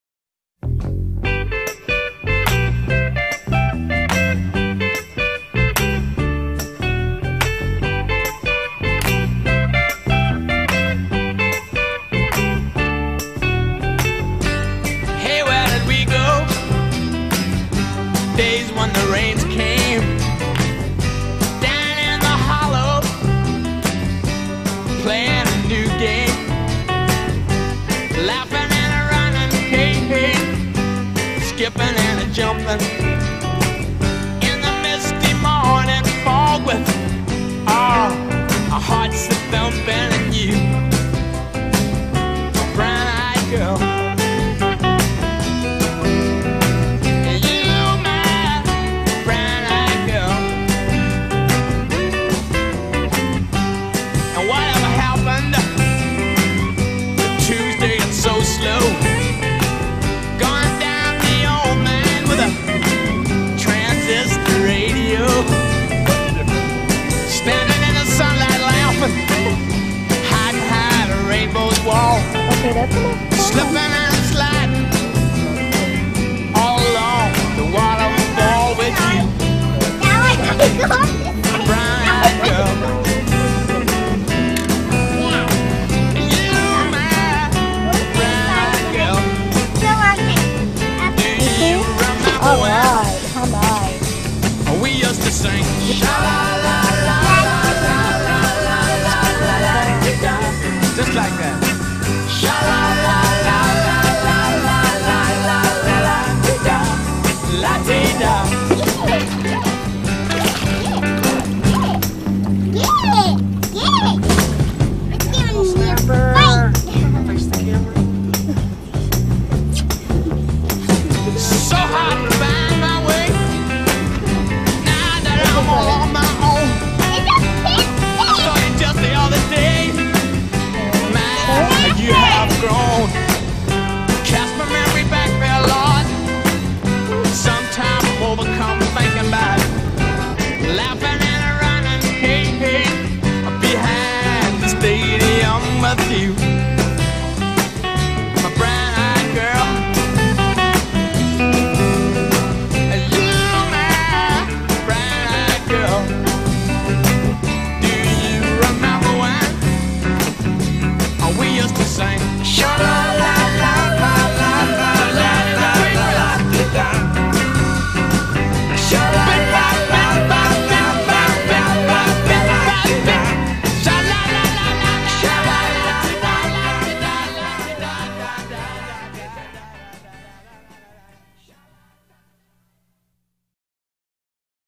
Mangrove Snapper at Sebastian
A day of fishing the grass flats in the Indian River Lagoon near Sebastian, Florida.